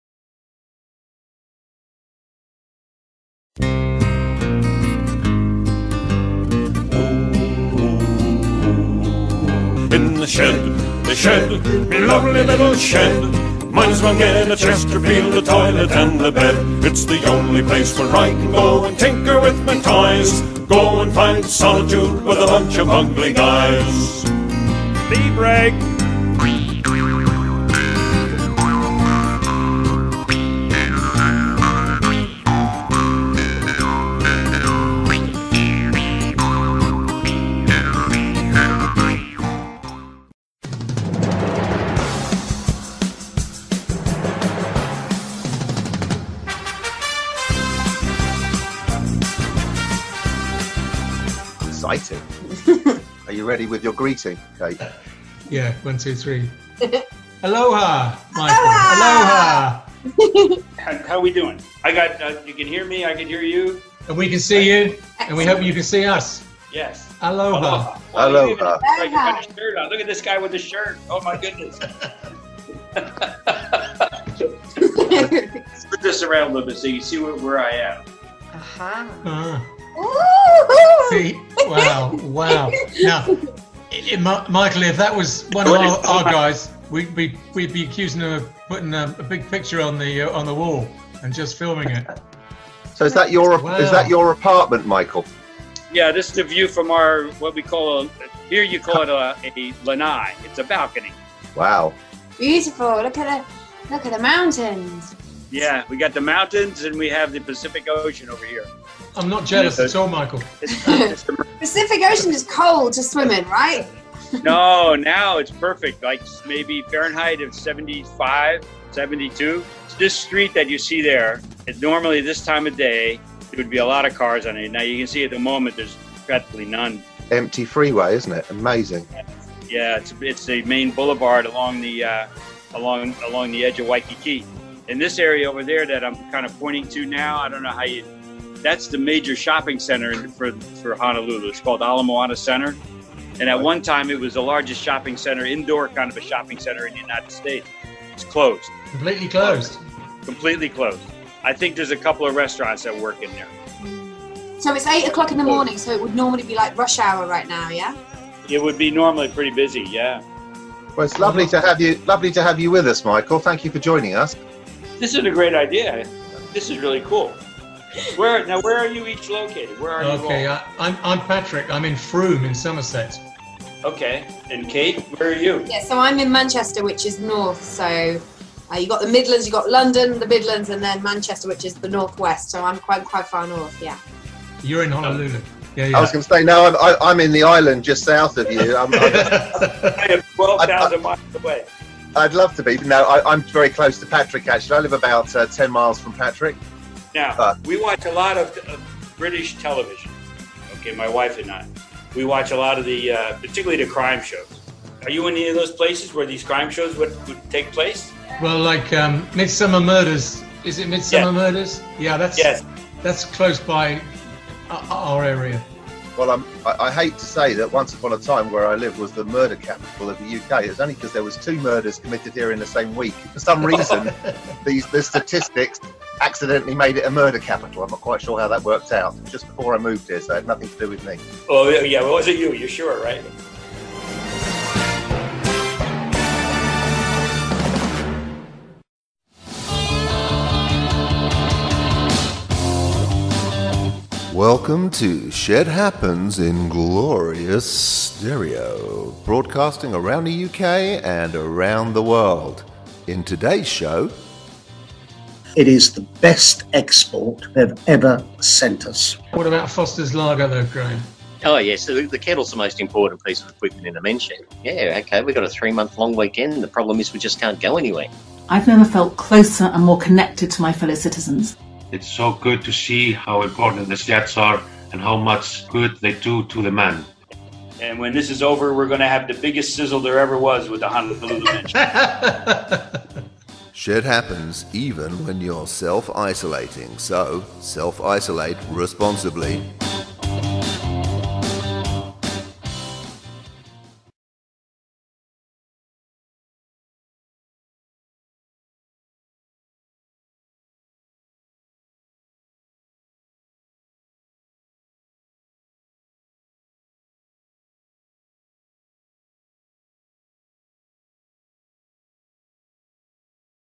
Shed Happens The Movie
The UK Shed Show is connecting people around the UK and around the world!  We are not sure how many shows there will be yet but here is some great insider footage and audio from (INTERNATIONAL) Show 3

Always 'Shed' and Self Isolate Responsibly!